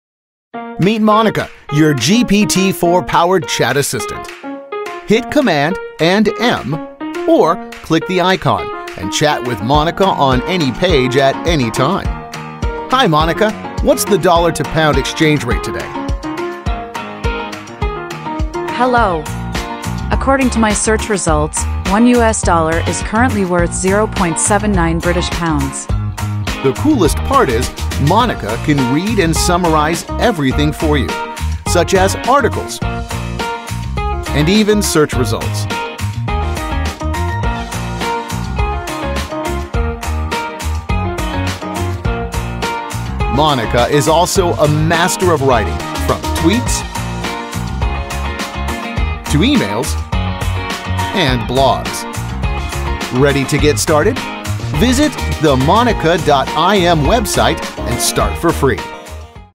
Monica - Your GPT-4 AI Assistant Chrome Extension
Monica is an extension powered by ChatGPT API that can chat with you about anything, generate copywriting using over 80 templates, and translate, rephrase, and explain text on any web page. It's available on all websites and can be accessed by hitting Cmd+M.